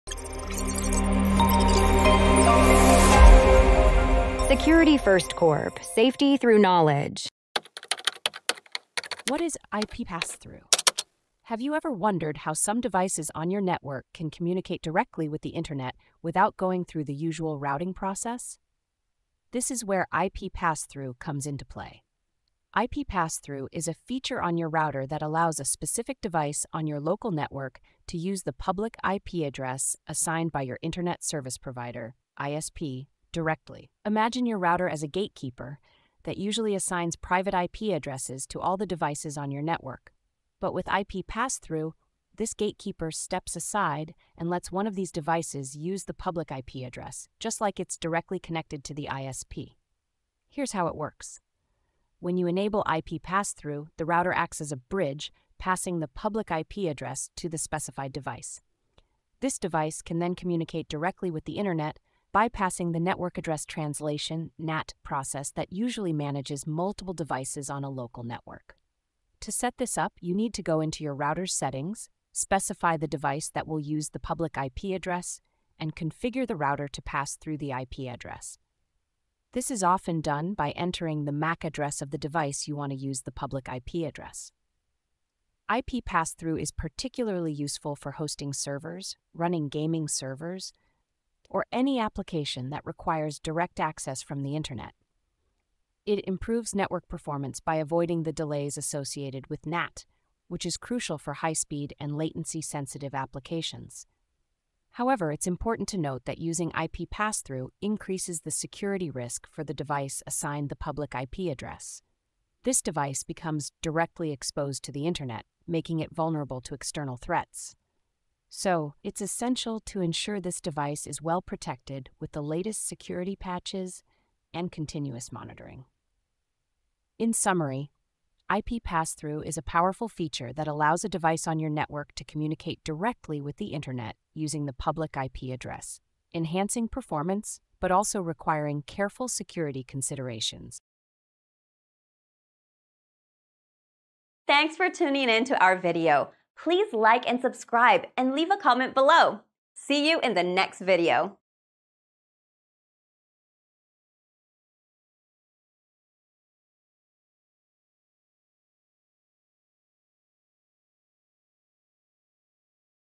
What Is IP Passthrough? - SecurityFirstCorp.com
What Is IP Passthrough? IP passthrough is a feature that allows a device on your network to connect directly to the internet using the public IP address assigned by your Internet Service Provider (ISP). In this video, we will cover the essentials of IP passthrough, including how it works and the benefits it brings to your network configuration. We will explain the process of enabling IP passthrough on your router and detail the steps needed to specify which device should use the public IP address. This feature is particularly beneficial for those hosting servers or running applications that demand direct internet access.

 While IP passthrough can improve performance and reduce latency for critical applications, it also poses security risks. We will address the importance of protecting the device that is assigned the public IP address and provide tips on how to secure it against potential threats. Understanding the balance between performance and security is key for anyone looking to utilize IP passthrough effectively.